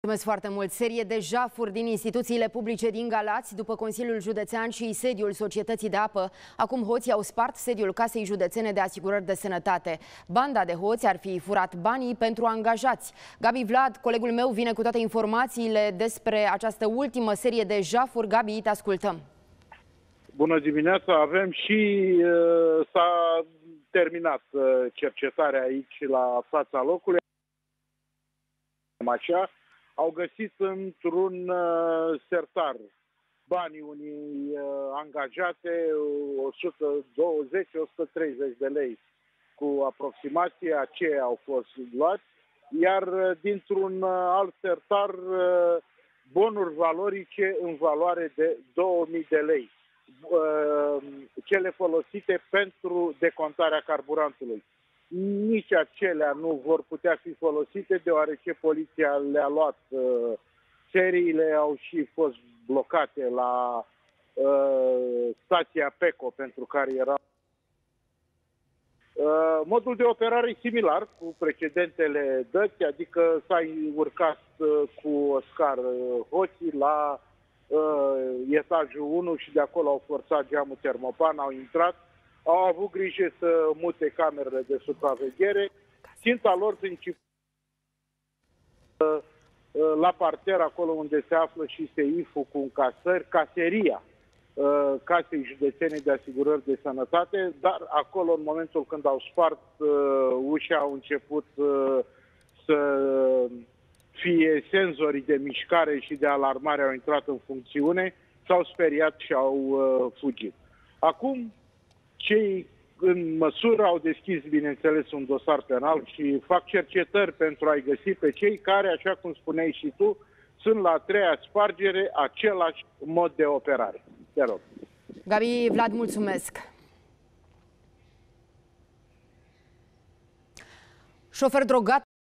Serie de jafuri în instiuţiile publice din Galaţi | Sediul CAS a fost prădat de hoţi
Serie de jafuri în instuţiile publice din Galaţi după Consiliul Judeţean şi sediul Societăţii de Apă, Hoţii au spart sediul Casei Judeţene de Aşigurări de Sănătate (CAS Galaţi).